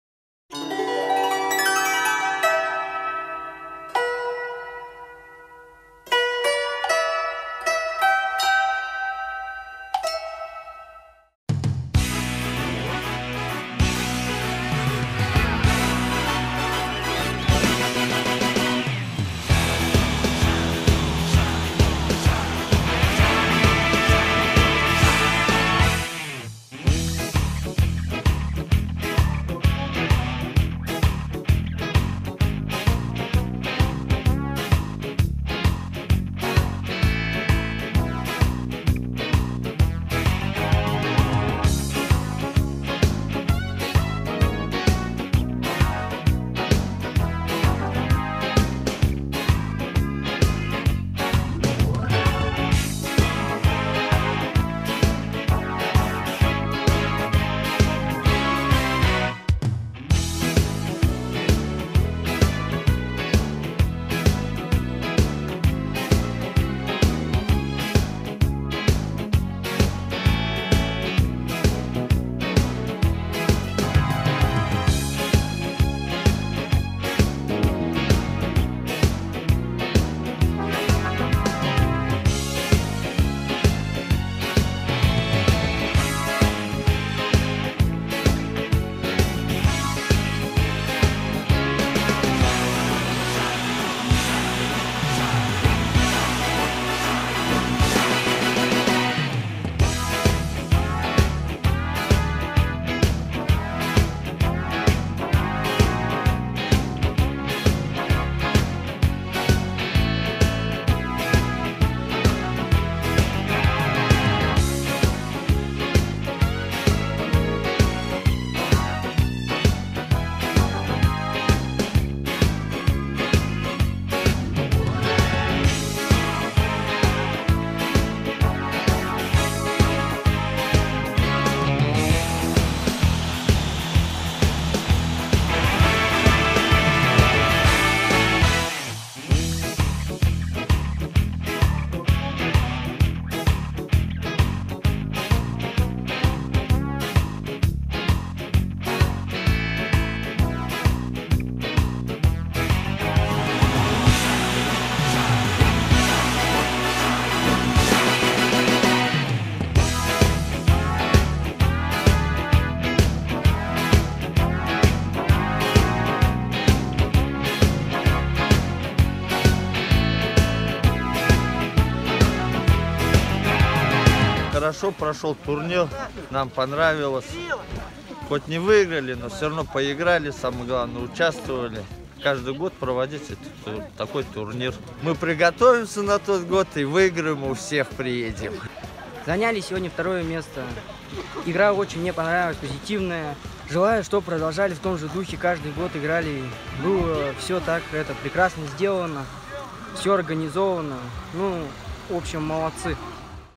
Кубок Даурского заповедника по хоккею на валянках
На льду реки Онон, которая является одним из истоков великого Амура и домом для более чем двадцати видов рыб, в том числе из семейства лососесых, в минувшую субботу прошел турнир по хоккею на валенках на приз Даурского заповедника. Турнир был посвященном международному Году лосося. В нем приняли участие четыре команды из Ононского района: молодежные команды из Нижнецасучейской и Верхнецасучейской СОШ, разновозрастная команда из села Первый Чиндант и команда заповедника «Даурский».